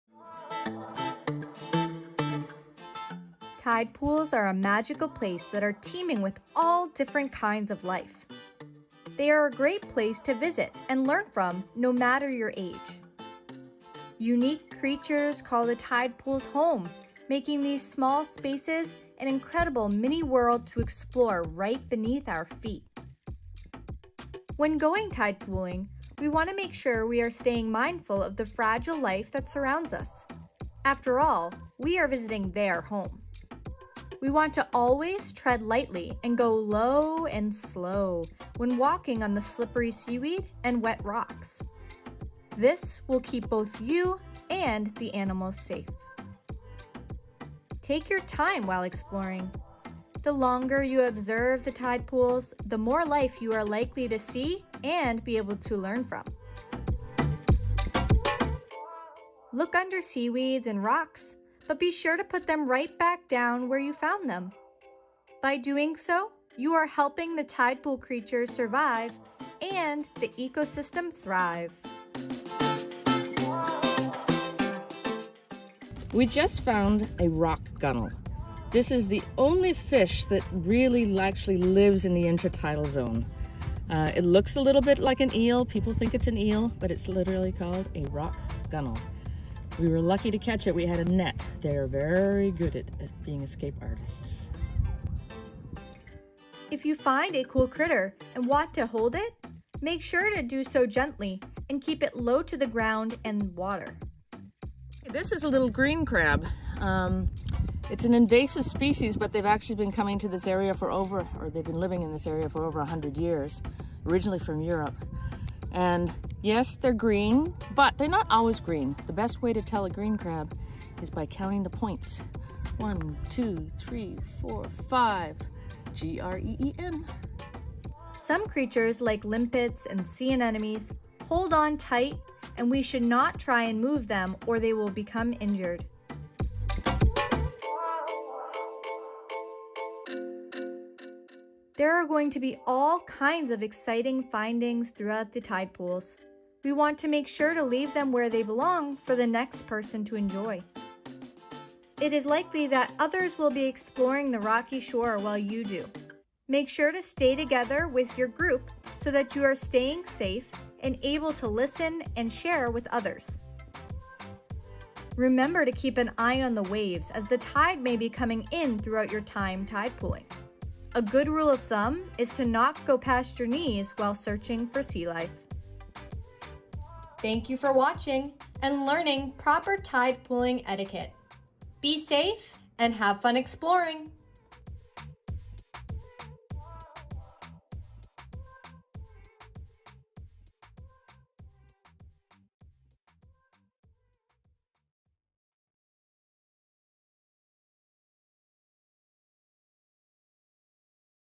Explore the Rocky Shore: Tide Pooling 101
The rocky shore is a unique habitat teaming with all different kinds of life! Algae and animal species call tidal pools "home," and is where you will find and be able to learn from these fascinating creatures!

In this video you will gain insight on what you may find inside the tide pools, what to do and look for during your time out on the rocks, and how to use proper tide pooling etiquette while exploring.

To learn more about the Tide Pools and to gain access to helpful resources and our Rocky Shore Field Guides,

For educators looking to add the Rocky Shore Ecosystem to your curriculum, visit our Rocky Shore Curriculum resource